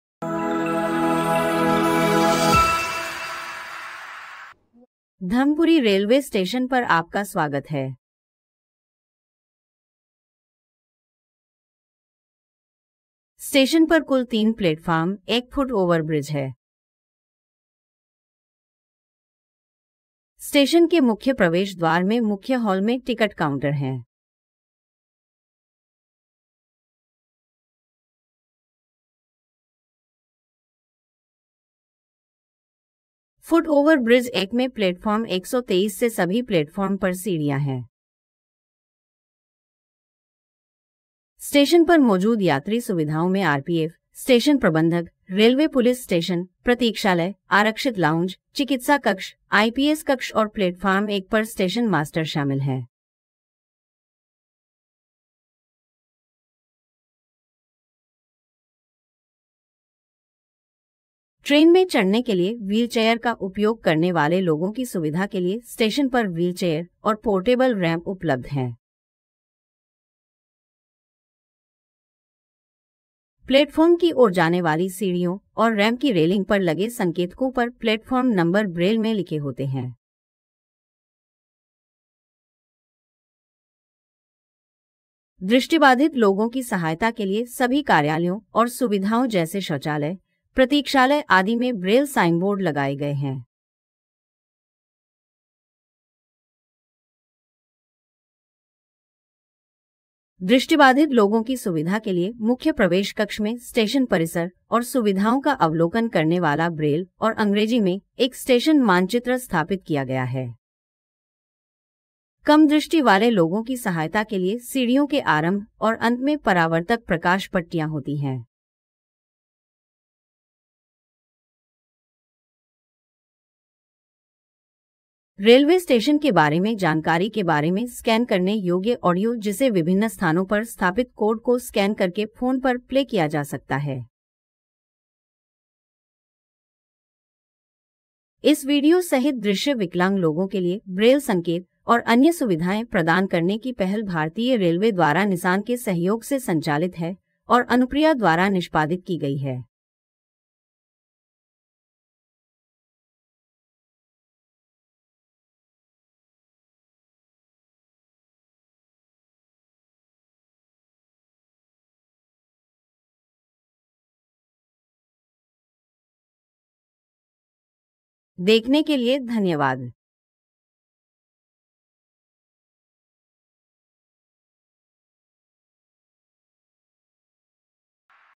धर्मपुरी | सांकेतिक भाषा में जानिए रेलवे स्टेशन के बारे में .
Link for English version
श्रवण बाधित लोगों के लिए सांकेतिक भाषा में धर्मपुरी रेलवे स्टेशन पर उपलब्ध सुविधाओं के बारे में एक संपूर्ण मार्गदर्शिका। हमारा वीडियो देखें और अपने स्टेशन के बारे में जानें। विकलांग लोगों के लिए स्टेशन परिसर में मौजूद प्लेटफार्मों, फुट-ओवर ब्रिज, टॉयलेट, रिटायरिंग रूम, प्राथमिक चिकित्सा, क्लोकरूम आदि की संख्या और पहुंच सुविधाएं।

हम विकलांग लोगों के लिए यात्रा को सुलभ और सुविधाजनक बना रहे हैं। यह वीडियो भारतीय रेलवे के सहयोग से निसान इंडिया की सीएसआर पहल के हिस्से के रूप में बनाया गया है। इस परियोजना के लिए परियोजना निष्पादन भागीदार अनुप्रयास है। अनुप्रयास सभी सार्वजनिक स्थानों को सभी विकलांग लोगों के लिए सुलभ बनाने के लिए काम करता है। हम आपके लिए और आपके साथ काम करने में विश्वास करते हैं।